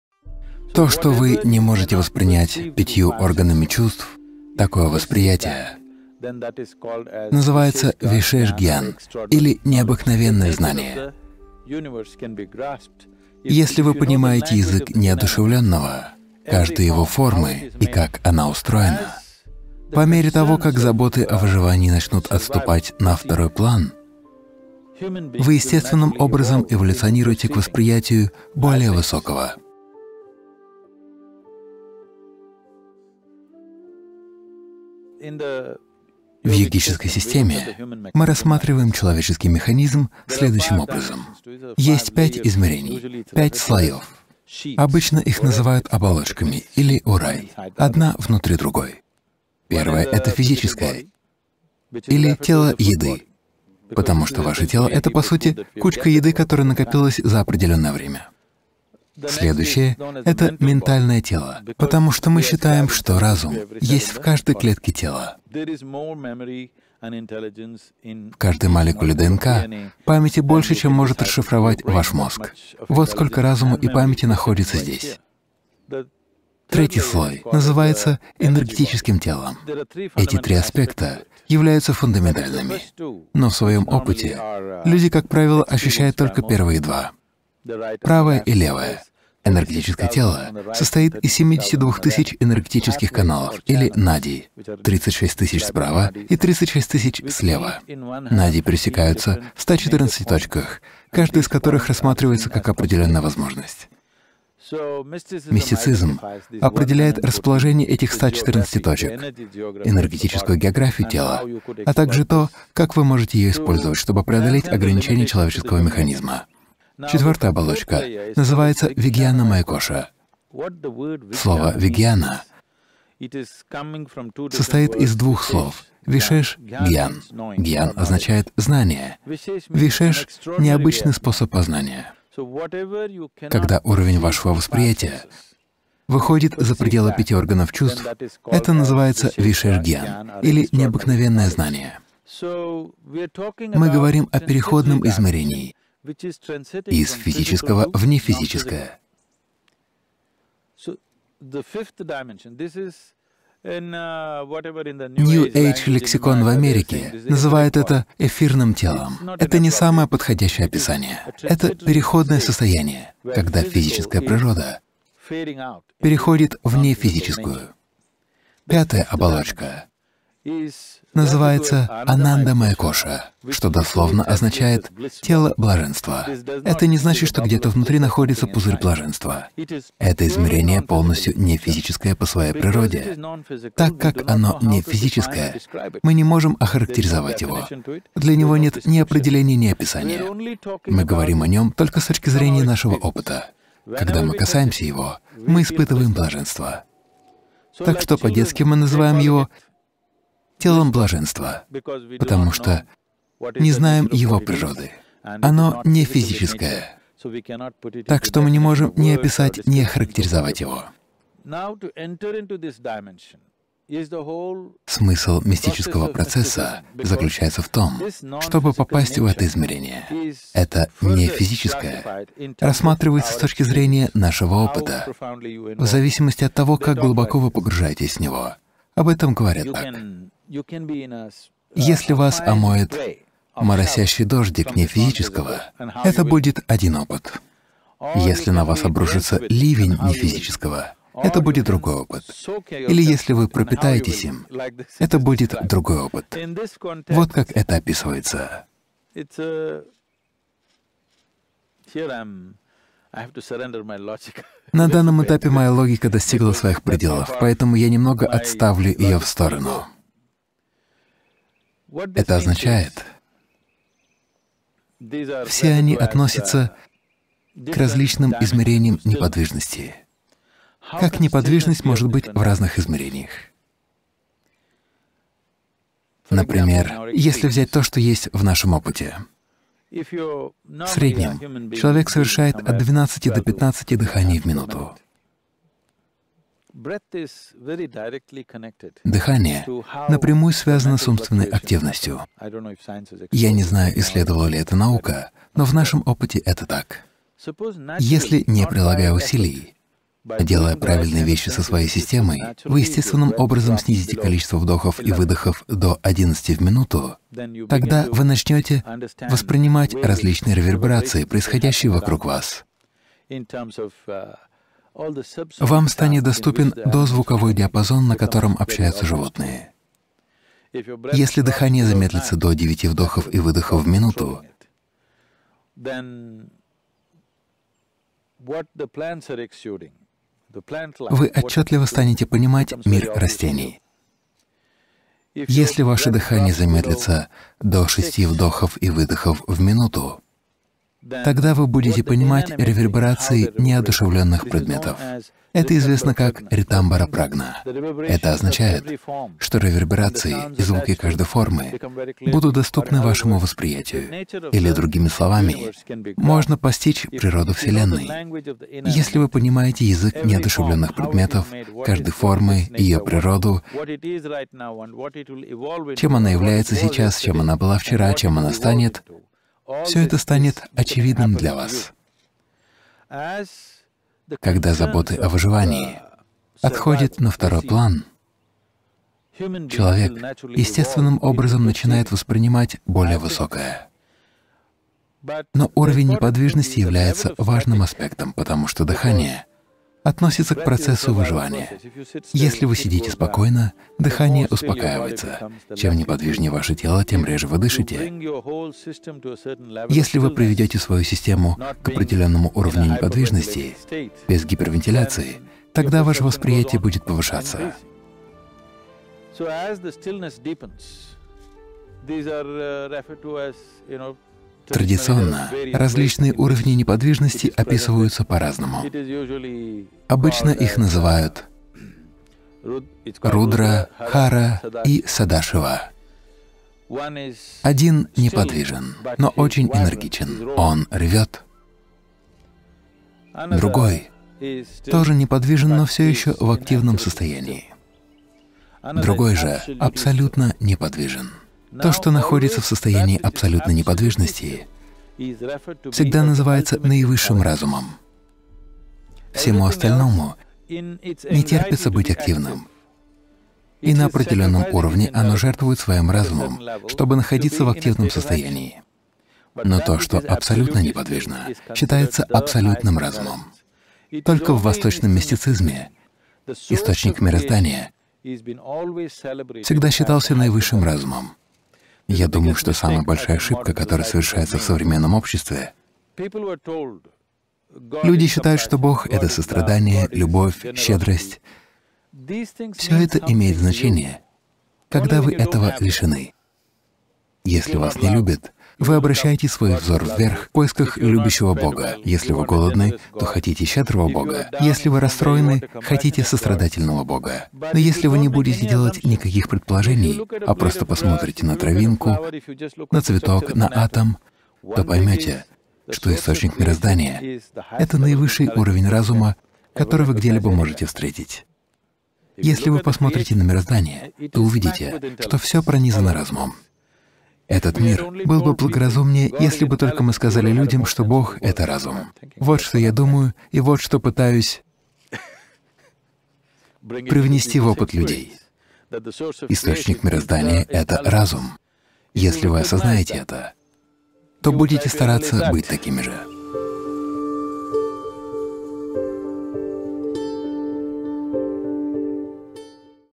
За пределами 5 органов чувств
Как мистики узнают секреты Вселенной? Какие измерения доступны человеку за пределами 5 органов чувств? В новом видео Садхгуру рассказывает, что дыхание и неподвижность – это ваш мост из физического в нефизическое измерение жизни.